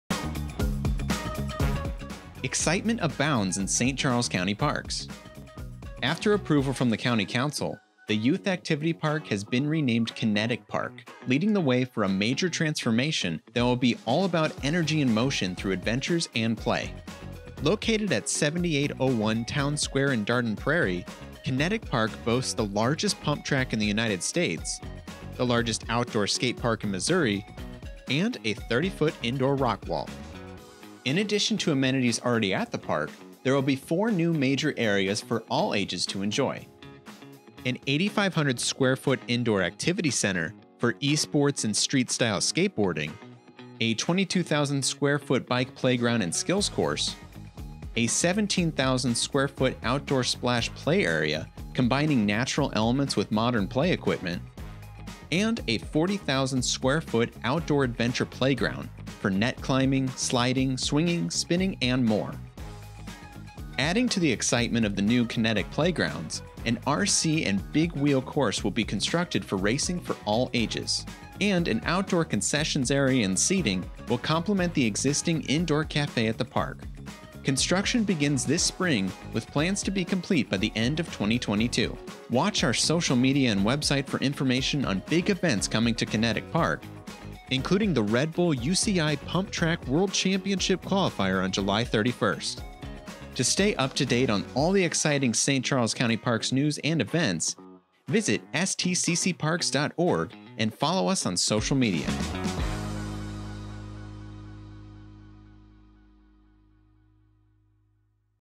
St. Charles County Youth Activity Park Renamed Kinetic Park; Major Transformation, Events Planned
The approval by the St. Charles County Council to change the name of Youth Activity Park to Kinetic Park leads the way for a major transformation that will be all about energy in motion through adventures and play. Construction will begin this spring at the park in Dardenne Prairie, and all elements of the transformation are expected to be complete by the end of 2022.